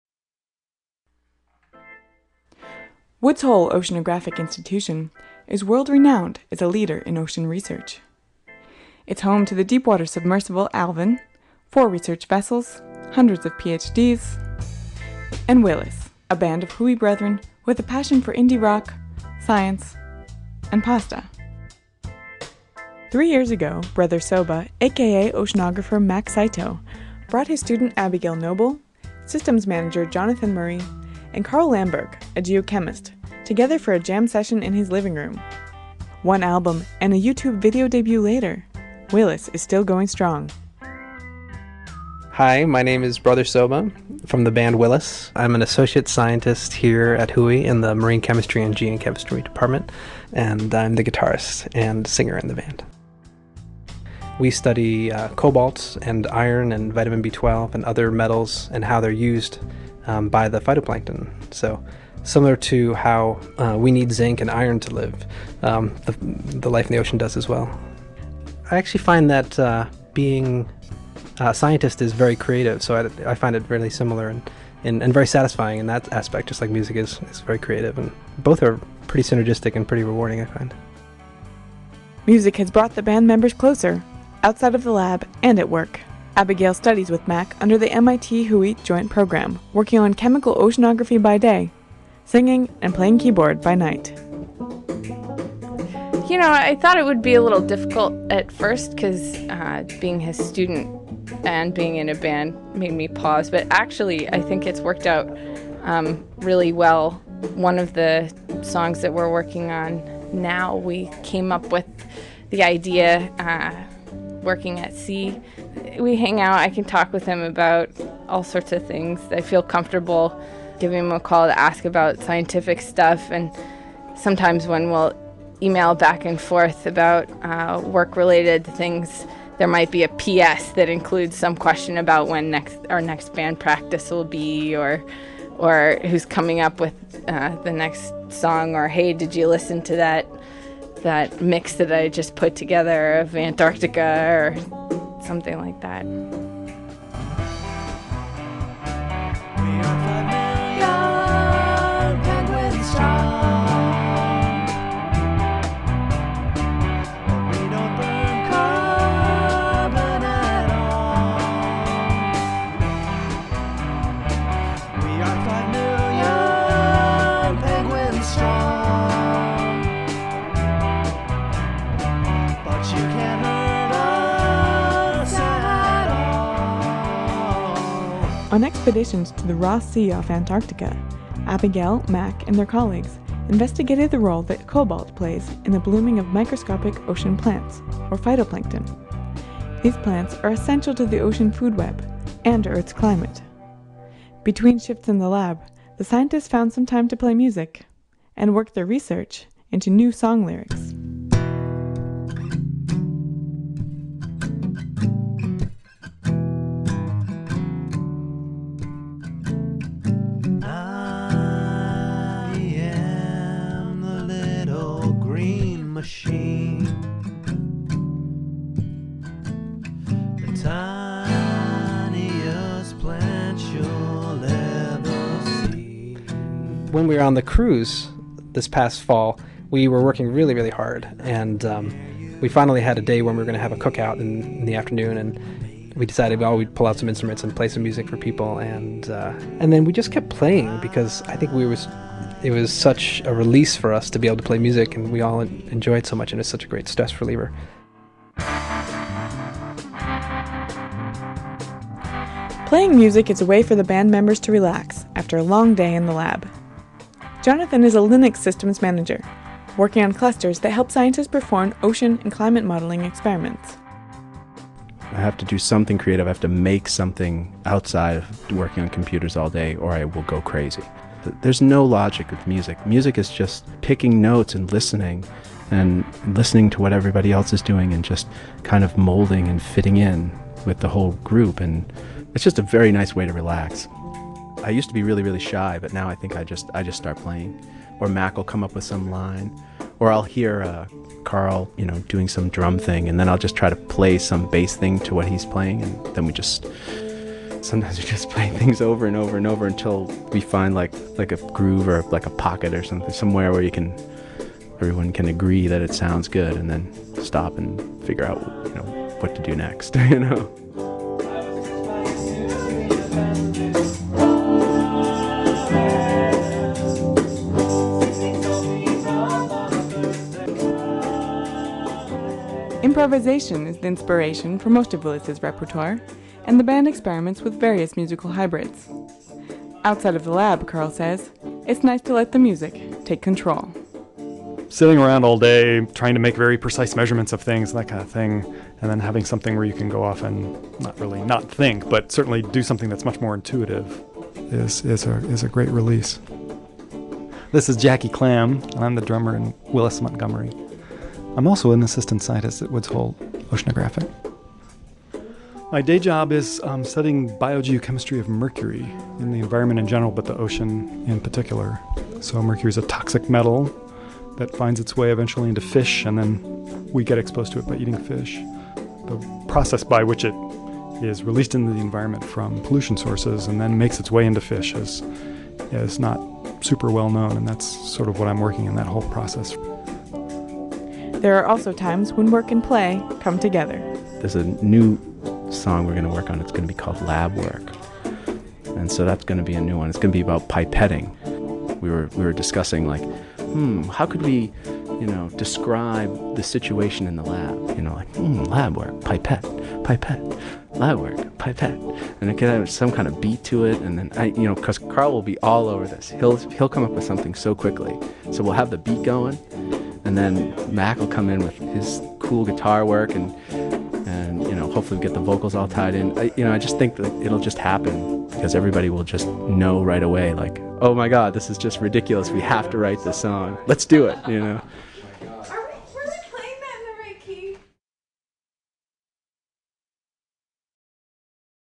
Researchers Band Together to Create a Band
Four researchers at Woods Hole Oceanographic Institution mix music into their lives and, occasionally, science into their lyrics.